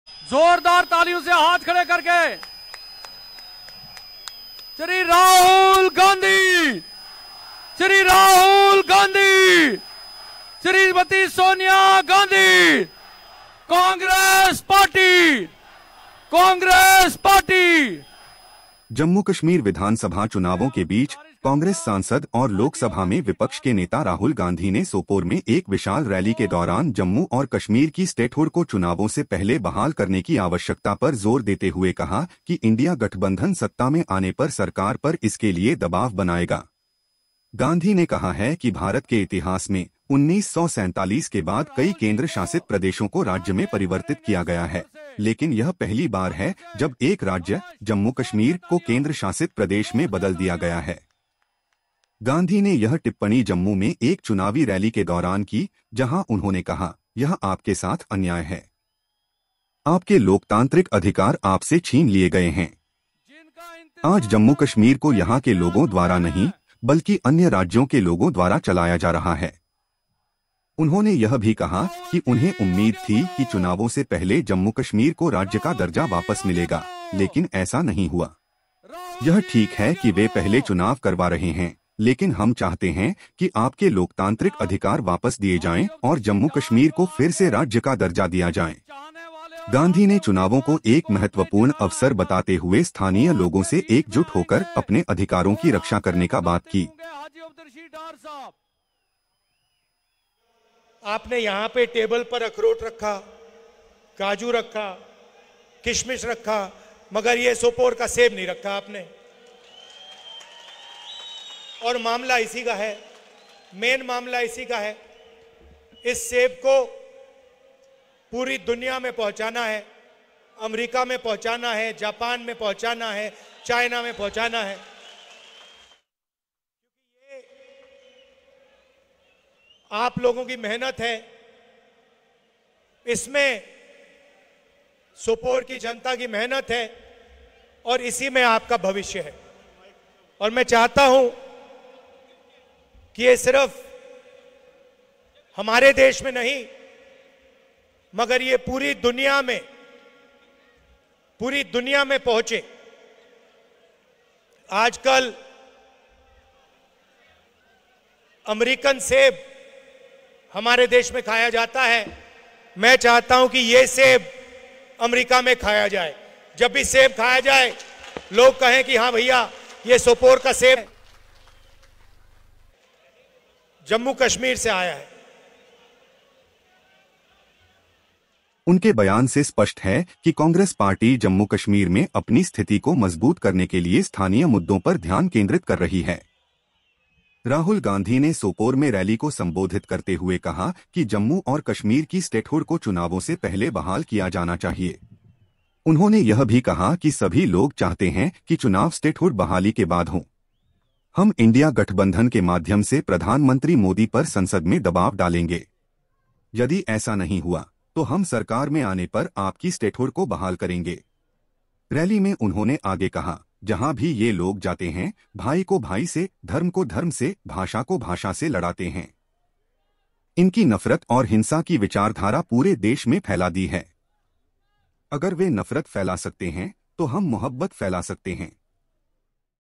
जम्मू-कश्मीर के सोपोर में गरजे राहुल गाँधी, बोले सत्ता में आयेंगे तो पूर्ण राज्य का दर्जा दिलवाएंगे!
Watch Rahul Gandhi addresses the public in Sopore, Jammu & Kashmir | Sopore | INCJammuKashmir